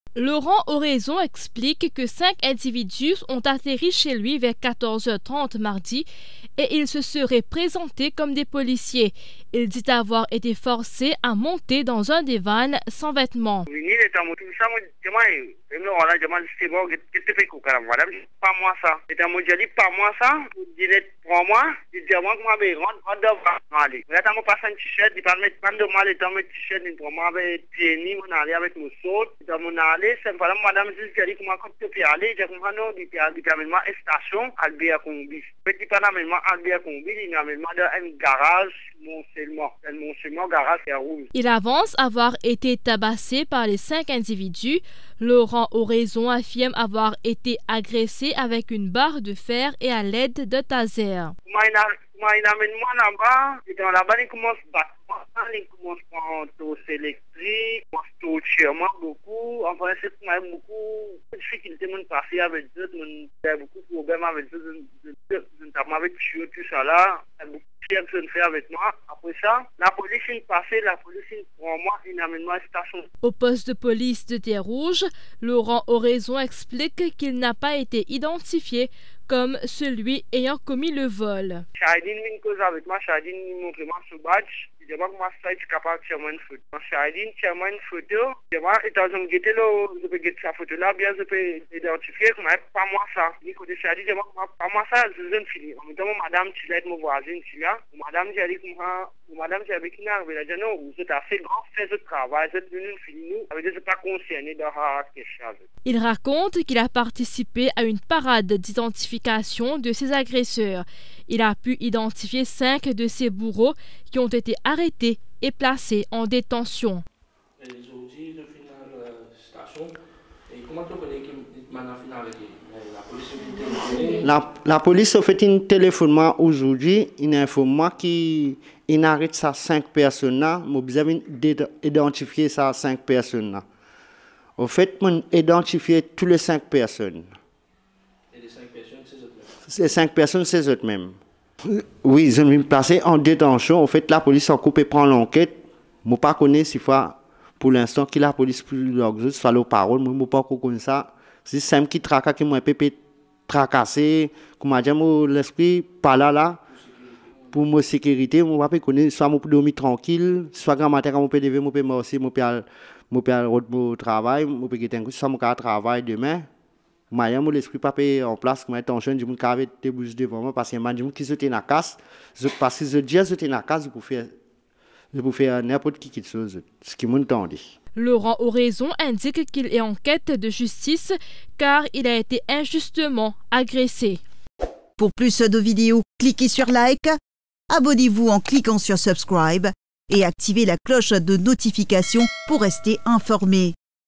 Allégation de séquestration à Terre Rouge
Allégation de séquestration à Terre Rouge : Laurent Oraison, un habitant de Cité la Cure, ‘j’ai été forcé à monter dans le van sans vêtements, j’ai été agressé avec une barre de fer et à l’aide d’un taser’, raconte-t-il